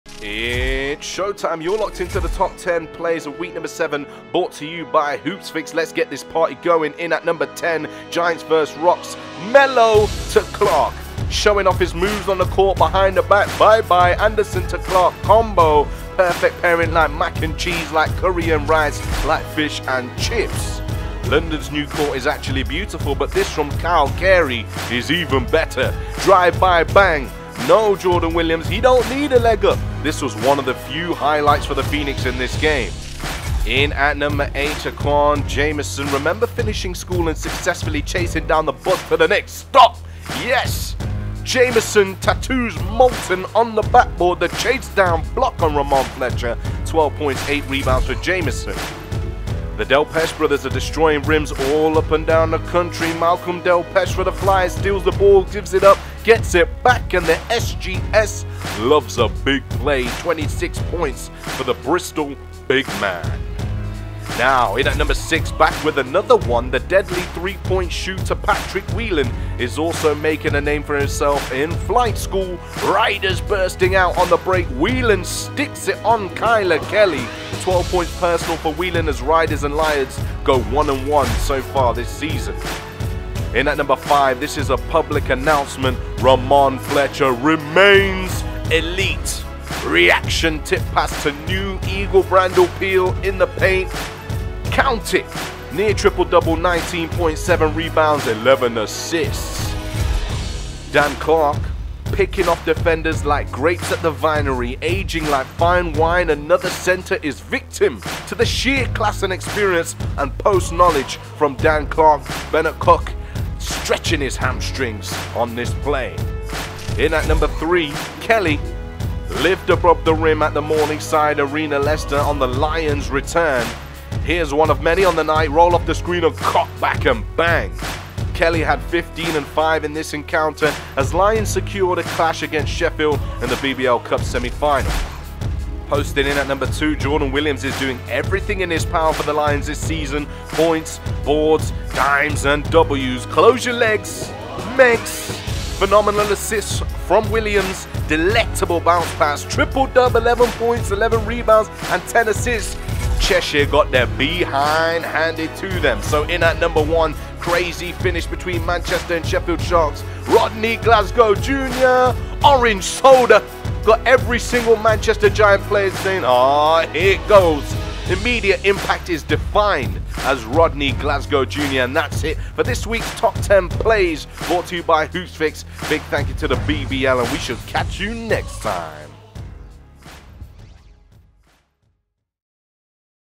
Rodney Glasgow Jr wins it at the buzzer! BBL Top 10 Plays - Week 7
Take a look back on the top 10 plays from Week 7 of BBL action, led by Rodney Glasgow Jr winning it for Sheffield at the buzzer!

Also featuring: Jamell Anderson, Dan Clark, Kyle Carey, Saiquan Jamison, Malcolm Delpeche, Patrick Whelan, Rahmon Fletcher, Kylor Kelley, Jordan Williams.